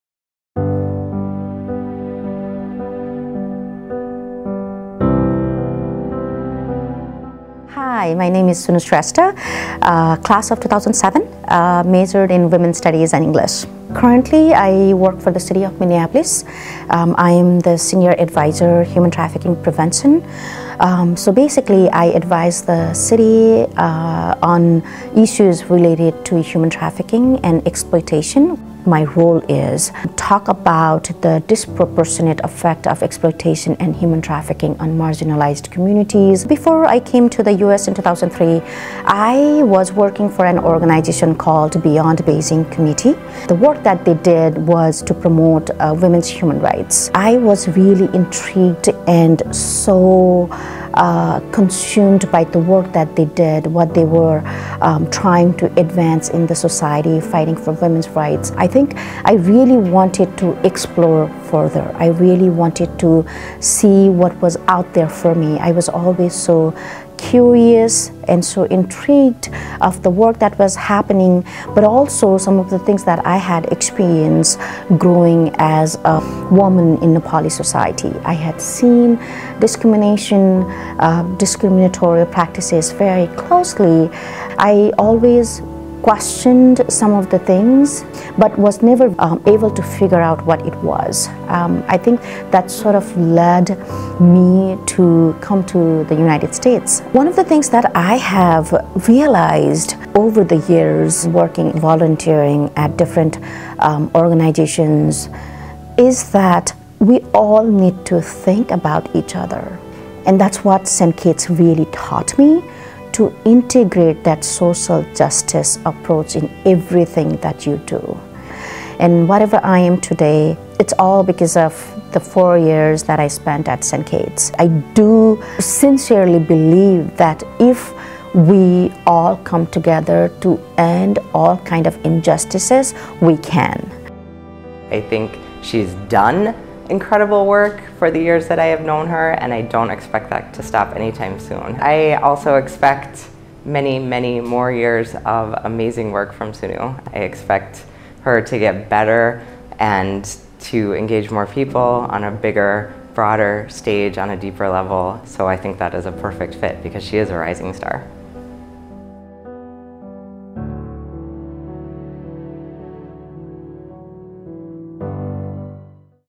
2019 Rising Star Award Winner: Shunu Shrestha
Each year, St. Kate's recognizes graduates who demonstrate excellence in leadership and service to others. We are proud to share that Shunu Shrestha '07 is a 2019 awardee.